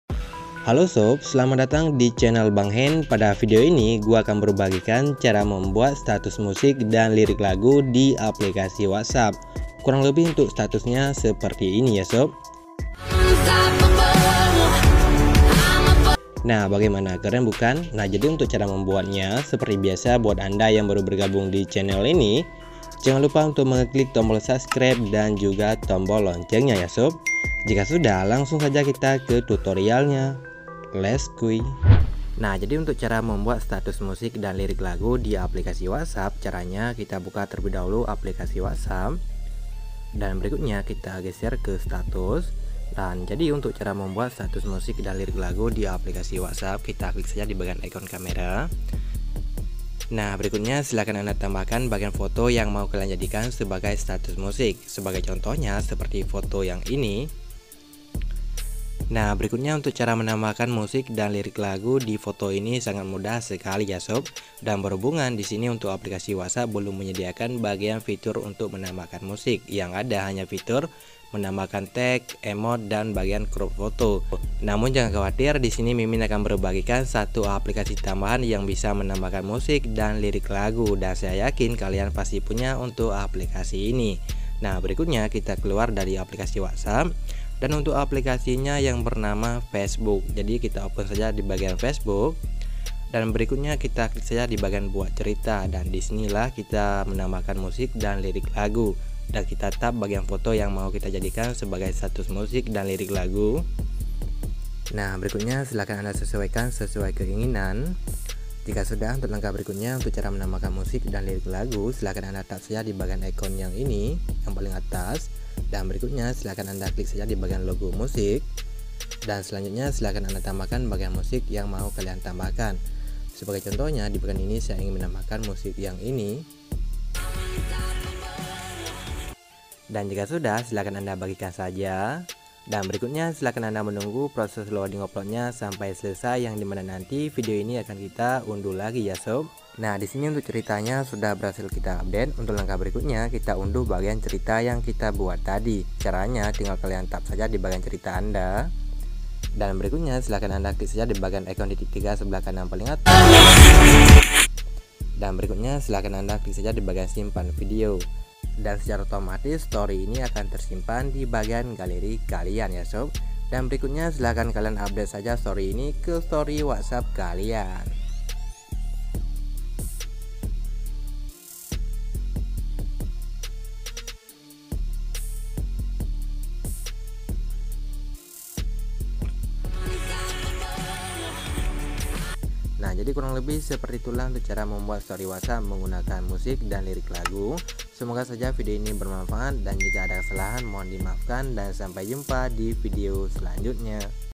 Cara Membuat Status Musik di Whatsapp 2022 Foto dan Lirik Lagu
Cara Membuat Status Musik dan Lirik Lagu di Whatsapp 2022

Video kali ini kita akan membahas tentang bagaimana Cara Membuat Status Musik dan Lirik Lagu di Whatsapp 2022

Dengan Membuat Status Musik di whatsapp pasti akan lebih seruuu,dan juga status musik yang kita buat akan di simpan oleh banyak kontak whatsap,selain itu juga banyak kelebihanya.

Mengirim status musik di whatsapp memang tidak di sediakan oleh fitur whatsapp itu sendiri, dan kita bisa saja mengirim status musik pada whatsapp tetapi dengan beberapa cara yang akan saya jelaskan dalam video ini

Bersama saya Bang ID, silahkan simak video tutorialnya

Music
Libray youtube